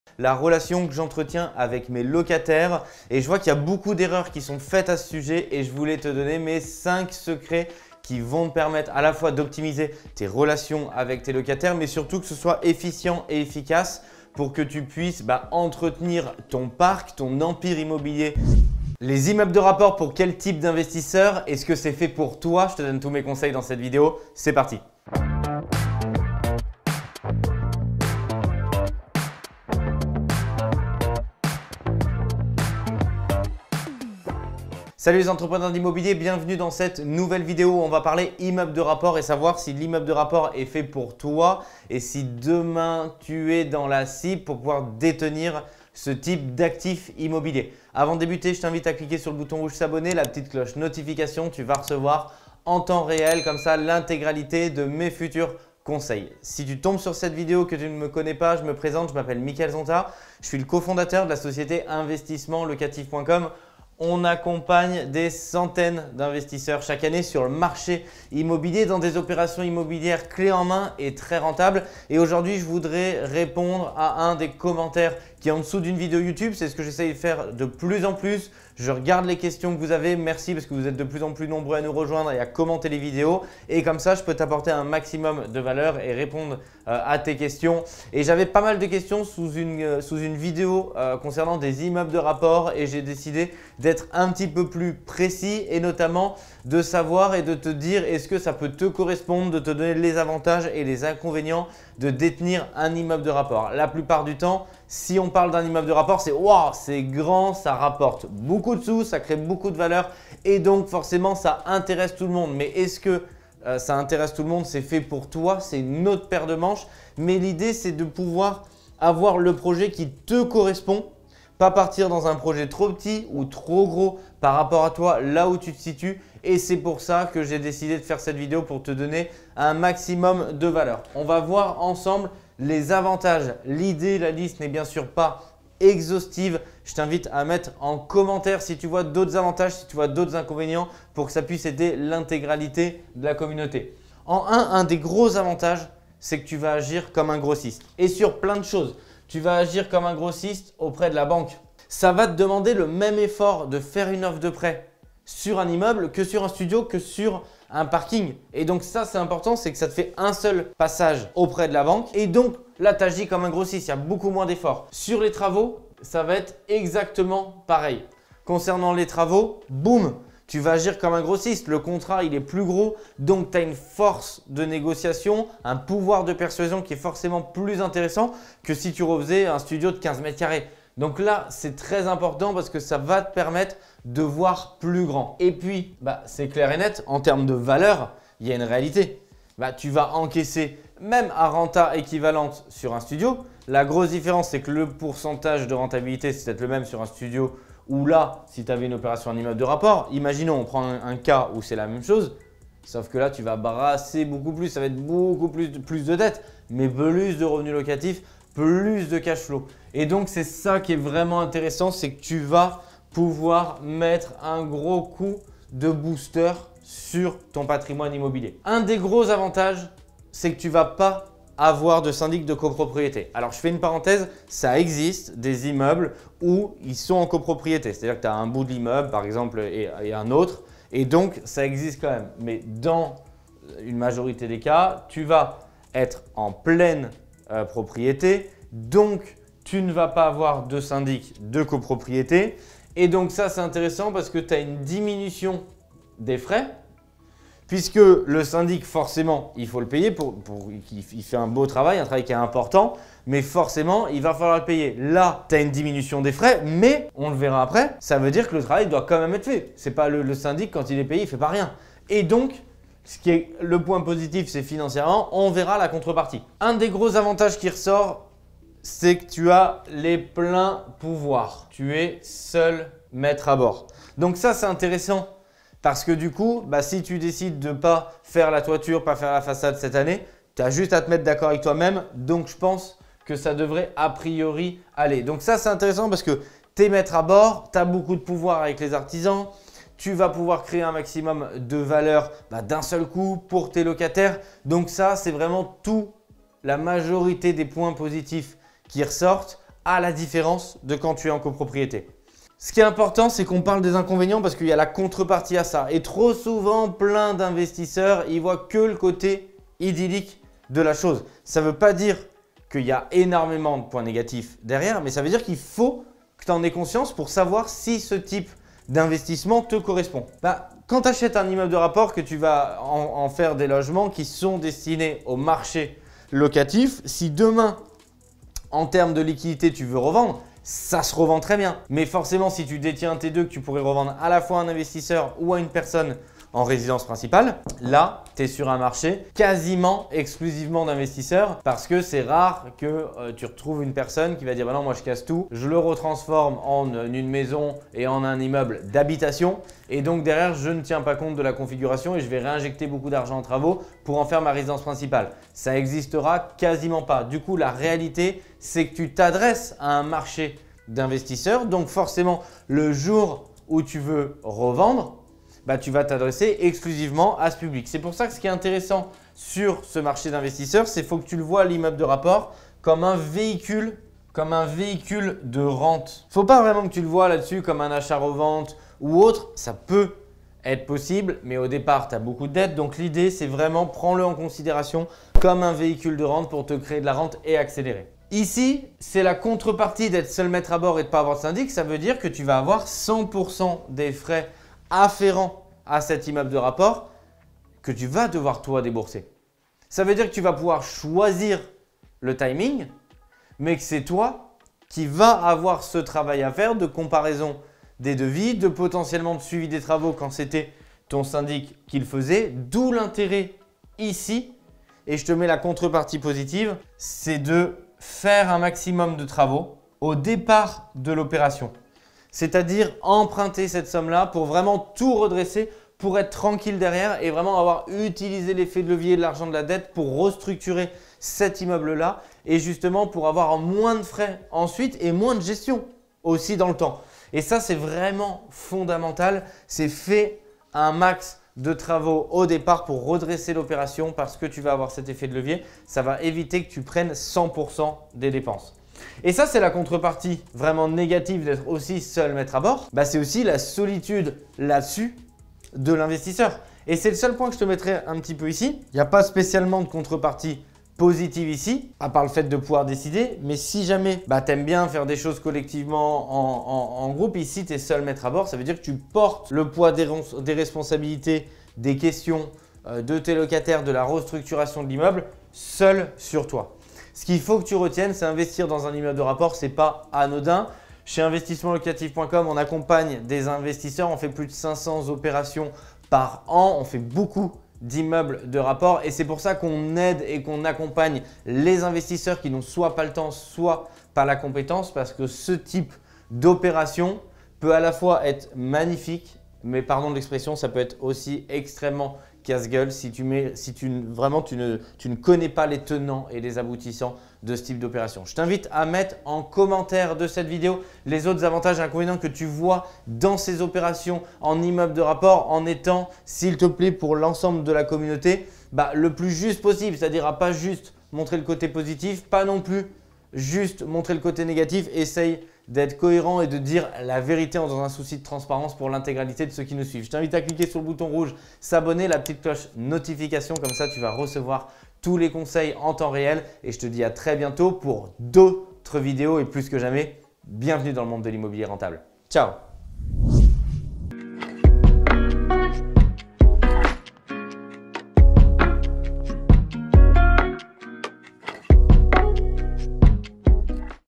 Immeuble de rapport : Avantages et inconvénients
COMMANDEZ votre exemplaire du livre de Mickael: Vive l'Immobilier, Vive la rente 🚀

En matière d’investissement immobilier locatif, l’achat d’un immeuble de rapport est souvent décrit comme une opération très avantageuse par rapport à différents achats séparés. Il est vrai que ce type d'investissement apporte de nombreux bénéfices, mais il ne faut pas négliger pour autant les inconvénients à prévoir.

Je partage avec vous dans cette video les avantages et les inconvénients à investir dans un immeuble de rapport. Est-il possible pour tout investisseur d'investir dans un immeuble de rapport ?

Avantages :
1. Vous pouvez agir comme un grossiste auprès de la banque.
2. Pas de syndic de copropriété ce qui permet une diminution des frais
3. Vous serez seul maitre à bord de votre immeuble. Donc vous aurez le plein pouvoir.

Inconvénients :
1. Vous serez uniquement sur un marché d'investisseurs. Ce qui fait d'un immeuble de rapport un véhicule de vente.
2. Vous aurez la totalité des frais afférents à l'immeuble de rapport. C'est vous qui gérer tout, et cela peut être très couteux.
3. Vous êtes le seul maitre à bord. Ce qui veut tout simplement dire que vous portez à vous seul le poids des responsabilités.

Bon visionnage.